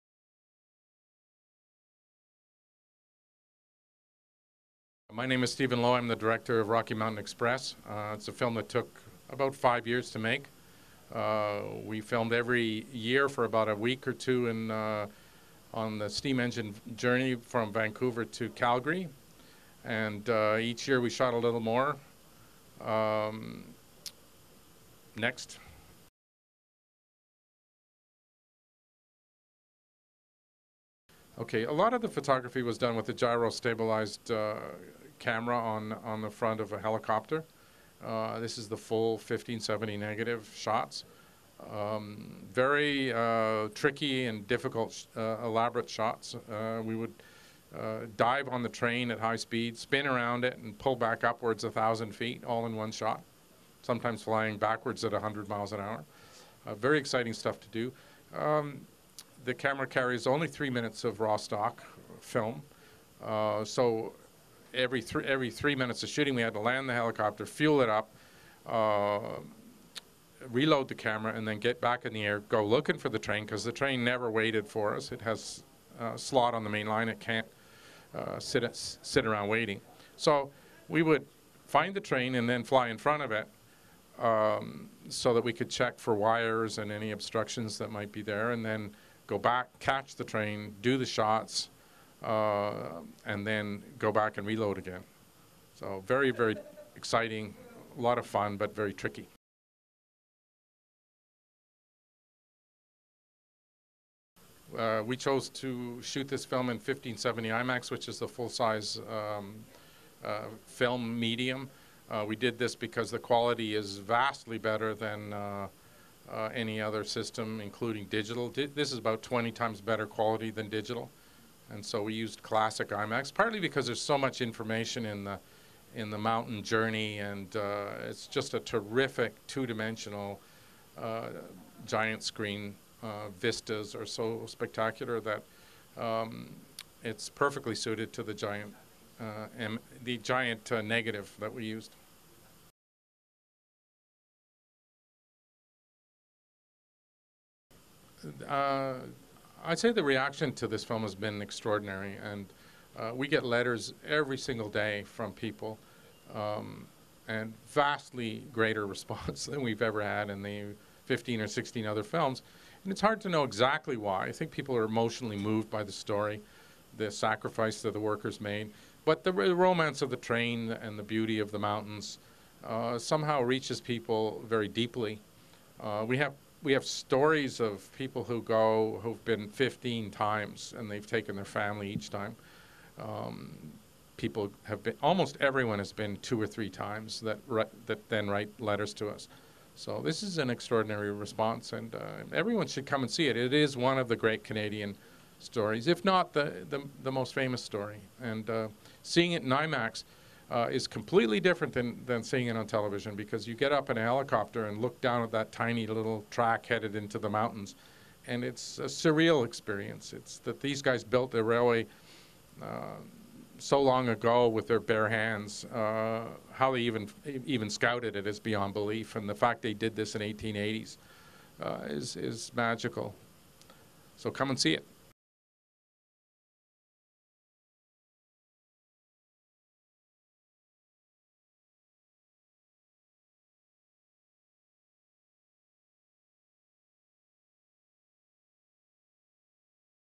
Entrevue avec Stephen Low, réalisteur de l'Express des Rocheuses
L'Express des Rocheuses est un magnifique film qui prend l'affiche au cinéma IMAX TELUS du Centre des sciences de Montréal dès le 5 octobre 2012